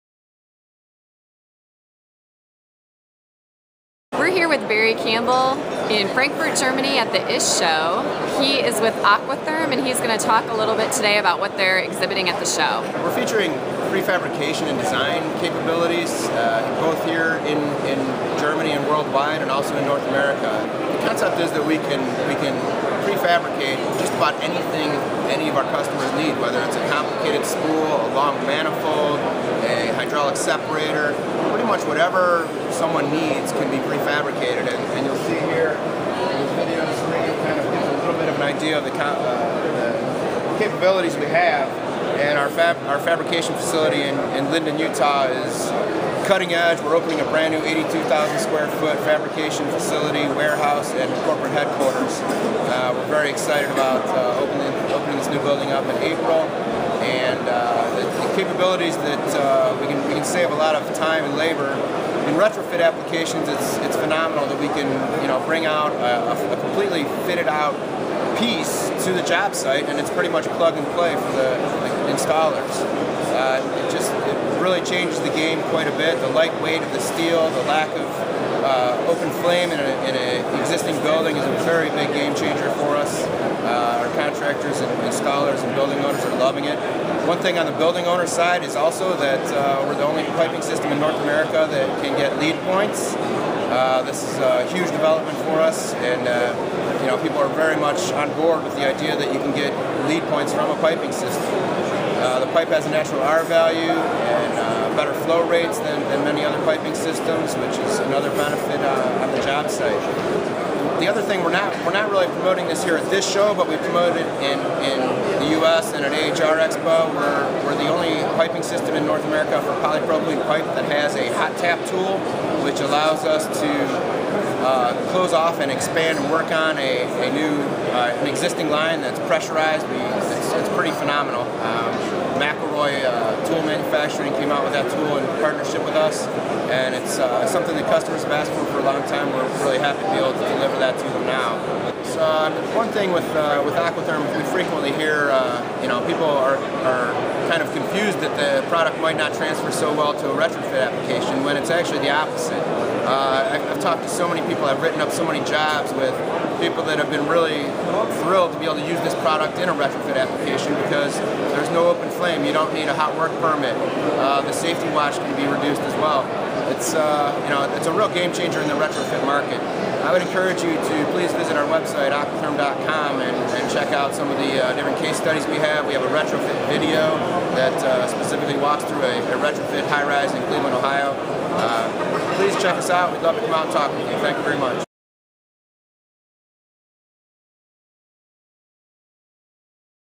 Aquatherm at ISH 2017
Barry Campbell, vice president of marketing for Aquatherm, explains some of the innovations Aquatherm has unveiled.